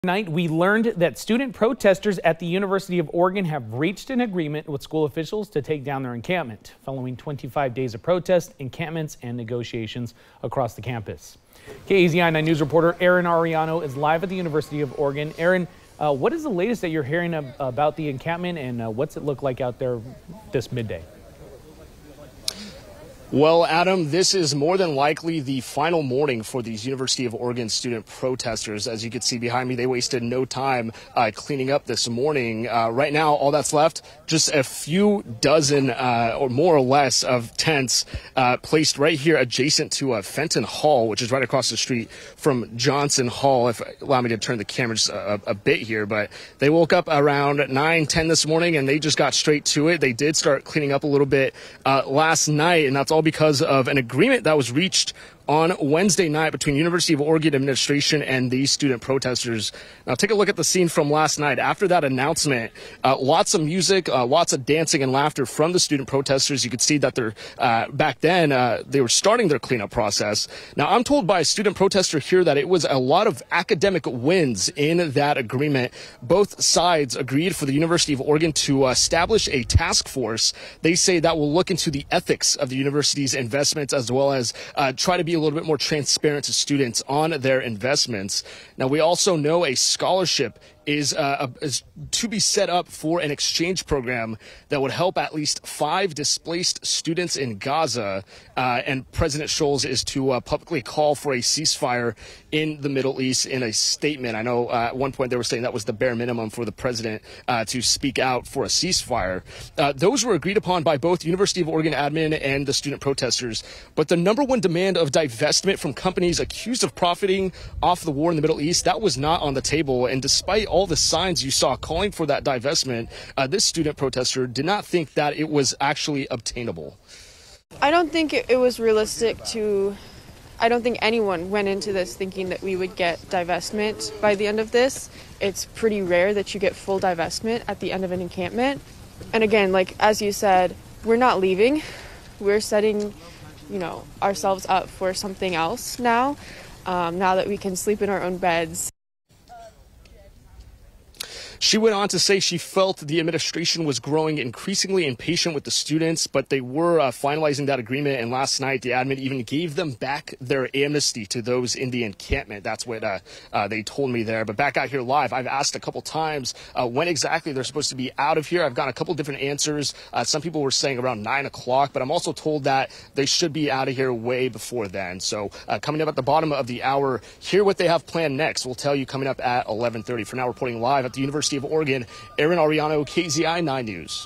University of Oregon, pro-Palestinian student protestors reach agreement to end encampment
Pro-Palestinian student protestors and University of Oregon administrators have reportedly reached an agreement to end the 25-day encampment.